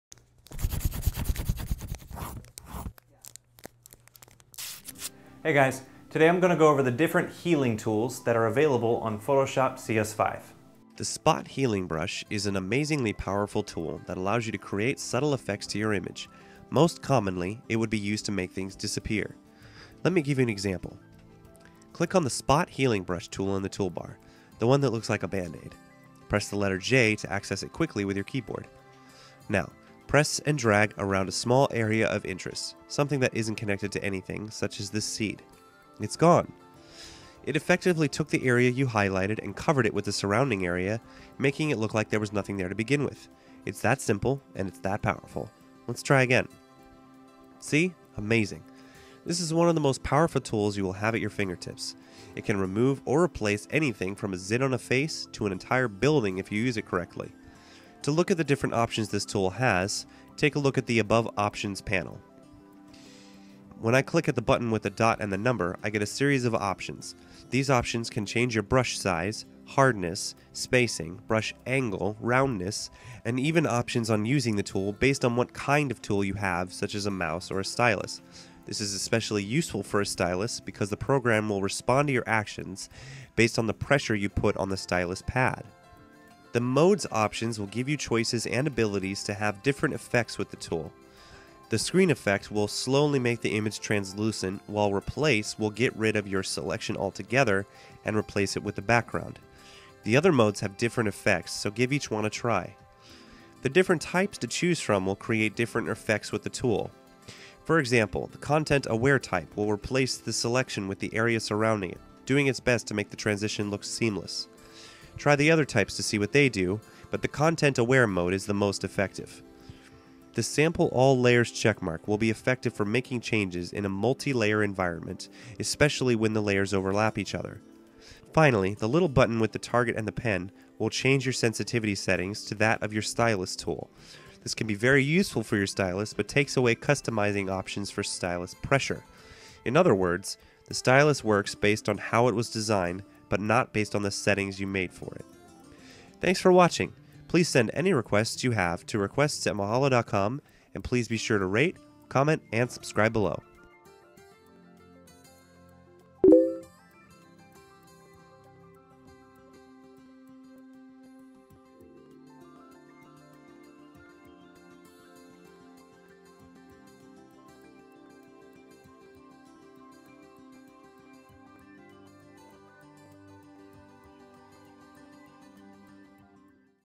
Learn Adobe Photoshop - Healing Brush Tool
In this video, Mahalo expert Justin Z. gives a rundown of what the Spot Healing Brush tool can do in Photoshop.

The Spot Healing Brush is an amazingly powerful tool that allows you to create and add subtle effects to your image.
Most commonly, it would be used to make things disappear. 1. Click on the Spot Healing Brush tool in the tool bar. It looks like a "bandaid." Press the letter J to access it quickly with your keyboard.2. Now, press and drag with your mouse around a small area of interest. Ideally, something that isn't "connected" to anything else in your image would be a good choice. 3. You'll notice that whatever you clicked over is now gone. This is because it effectively took the area you highlighted and covered it with the surrounding area, making it look like there was nothing there to begin with. The tool is that simple and that powerful. It can remove and replace almost anything, from a zit on a face to an entire building if you use it correctly. 4. To look at the different options this tool has, take a look at the above Options panel. When you click on the button with a dot and a number, you are given a series of options. These options can change your Brush Size, Hardness, Spacing, Brush Angle and Roundness. You're even given options on using the tool based on what kind of input device you have, such as a mouse or a stylus. This is especially useful for a stylus because the program will respond to your actions based on the pressure you put on the stylus pad.5. The Mode options will give you choices and abilities to have different effects with the tool. The Screen effect will slowly make the image translucent, while Replace will get rid of your selection altogether and replace it with the background. The other modes have different effects, so give each one a try. 6. The different Type to choose from will create different effects with the tool. For example, Content Aware type will replace the selection with the area surrounding it, doing its best to make the transition look seamless. Try the other Types to see what they do, but the Content Aware mode is the most effective. 7. The Sample All Layers checkmark will be effective for making changes in a multilayer environment, especially when the layers overlap each other. 8. Finally, the button with the "target and pen" will change your sensitivity settings to that of your stylus tool. This can be very useful for your stylus, but takes away customization options for stylus pressure. In other words, the stylus works based on how it was designed but not based on the settings you made for it.